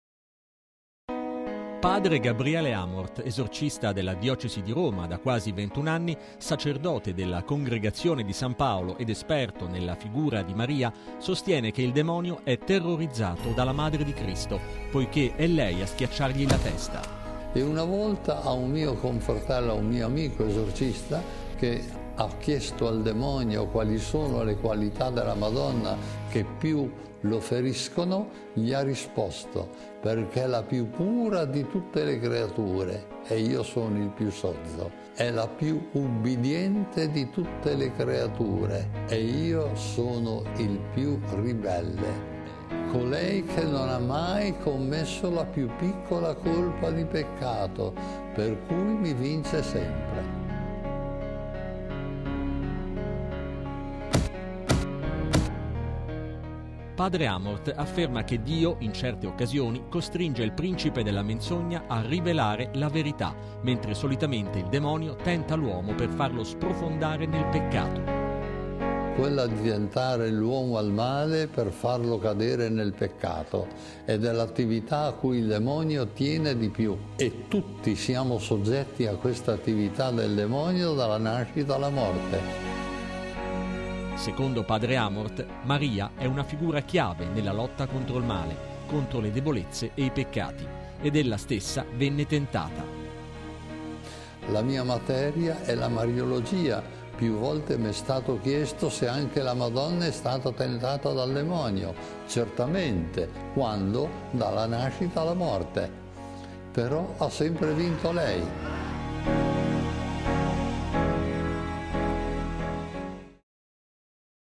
Maria Schiaccia Il Serpente
Padre Gabriele Amorth, esorcista della diocesi di Roma da quasi 21 anni, sacerdote della Congregazione di San Paolo ed esperto nella figura di Maria, sostiene che il demonio è terrorizzato dalla madre di Cristo poiché è lei a schiacciargli la testa. “In un’occasione un mio amico esorcista chiese al demonio che cos’era ciò che più lo feriva della Vergine, perché sentiva tanta avversione. E gli rispose: 'Perché è la più pura di tutte le creature ed io sono il più immondo, è la più obbediente di tutte le
creature ed io sono il più ribelle, è Colei che non ha mai commesso neanche il più piccolo peccato e per questo mi sconfigge sempre'”. Padre Amorth afferma che Dio, in
certe occasioni, costringe il principe della menzogna a rivelare la verità, mentre solitamente il demonio tenta l'uomo per farlo sprofondare nel peccato. “Portare l’uomo al male per farlo peccare. È l’attività che il demonio preferisce, e tutti ne siamo
soggetti dalla nascita alla morte”. Secondo padre Amorth, Maria è una figura chiave nella lotta contro il male, contro le debolezze e i peccati. Ed ella stessa venne tentata: “La mia materia è la mariologia e varie volte mi hanno chiesto se anche la
Madonna venne tentata dal demonio. Certamente. Quando? Dalla nascita fino alla morte. Ma vinse sempre”